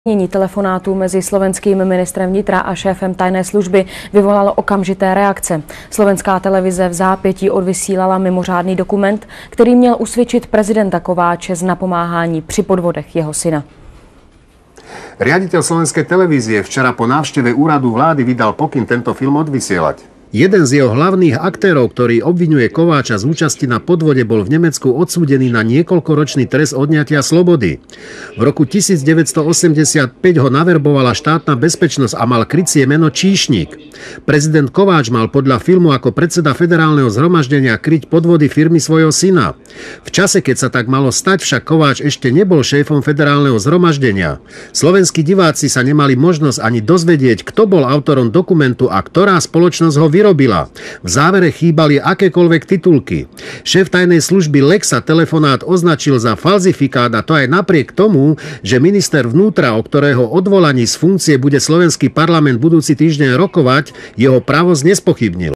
Zverejnenie telefonátu medzi ministrom vnútra a šéfom SIS, reportáž 15.05.1996
Zverejnenie telefonátu medzi ministrom vnútra Ľudovítom Hudekom a šéfom SIS Ivanom Lexom, reportáž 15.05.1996.
Rozhovor odvysielalo Rádio Twist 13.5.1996. Samotný telefonát je z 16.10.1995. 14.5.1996 o tom informoval aj denník SME. Odvysielanie Dokumentu v STV. Redaktor TV NOVA: Eugen Korda. Celé video